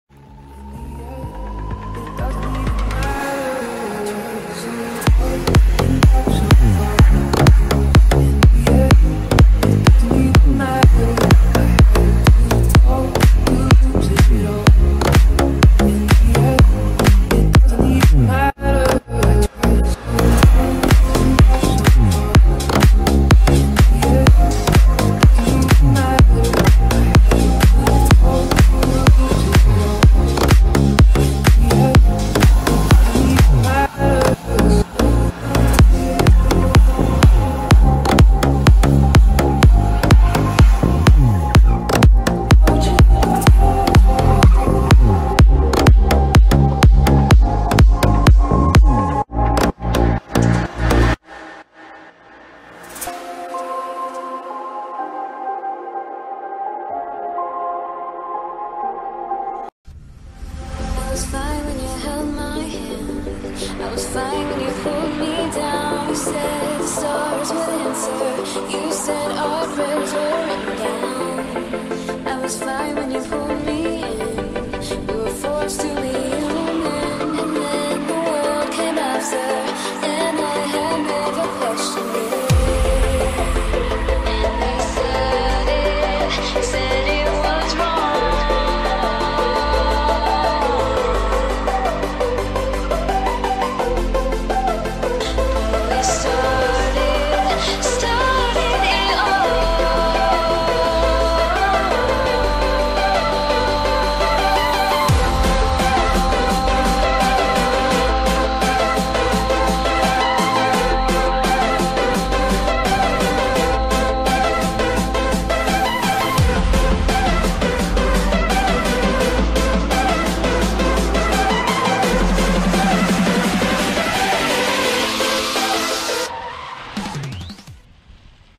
Drifting on Mercedes-Benz C-Class 2019 1.6T (156 hp) 9 automatic transmission
drift Drifting on Mercedes-Benz C-Class 2019 1.6T (156 hp) 9 automatic transmission